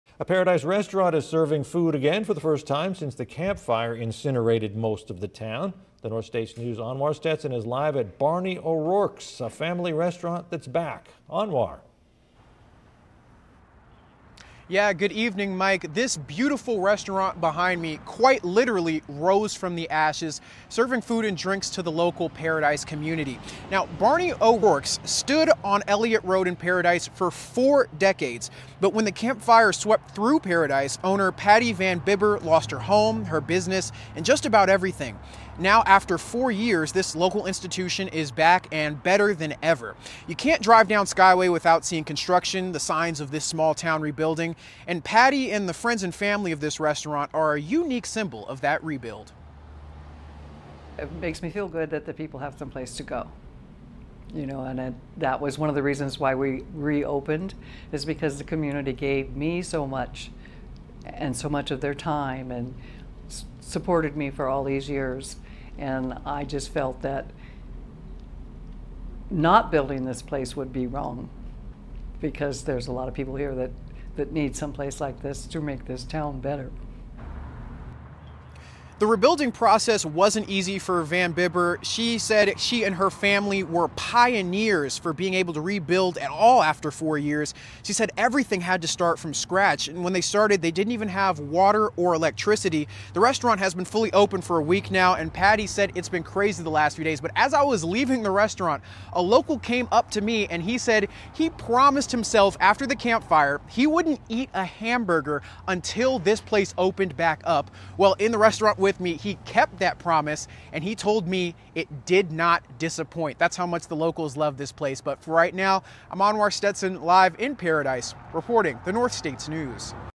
THIS BELOVED RESTAURANT IN PARADISE IS BACK FOUR YEARS AFTER THE CAMP FIRE
(ORIGINALLY AIRED NOVEMBER 28TH, 2022)
CREDITS:
STATION: KRCR NEWS CHANNEL 7
EDITOR: ANWAR STETSON
PRODUCER: ANWAR STETSON, MIRANDA ANGEL
WRITER: ANWAR STETSON
CAMERA: ANWAR STETSON
LIVE SHOT OPERATOR: ANWAR STETSON
ANCHOR: MIKE MANGAS

PARADISE, Calif. — Barney O’ Rourke’s is back on its feet four years after the Camp Fire. The local burger bar and grill literally rose from the ashes, serving food and drinks to its local Paradise community.

Patty Van Bibber and her family bought O’Rourke’s in 1981, moving to their current location at 740 Elliot Road in 2009. But when the Camp Fire swept through Paradise in 2018, Van Bibber lost her home, her business and just about everything.

But after four years, this local institution is back and better than ever.

You can’t drive down Skyway without seeing construction—the signs of this small town rebuilding. Patty Van Bibber, running the restaurant with her daughter, granddaughter, son and son-in-law, are a symbol of that rebuild.

“It makes me feel good that the people have someplace to go, and you know that was one of the reasons why we reopened,” Van Bibber said. O’Rourke’s has been open for a week, and she has described the last few days of business as “crazy”. “The community gave me so much and spent so much of their time supporting me for all these years, and I just felt that not building this place would be wrong. There’s a lot of people here that need a place like this to make this town better.”

The rebuilding process wasn’t easy for the Van Bibbers. Patty called her family “pioneers” for being able to start over. When they got back to Paradise, they had no water and no electricity to rebuild the restaurant.

However, Van Bibber was determined to continue. When asked when they first had the idea to bring the restaurant back, she said, “as soon as we saw the flames.”

One local walked out of the restaurant and said he had made a promise to himself not to eat a hamburger until O’Rourke’s came back. After four years, he said it did not disappoint.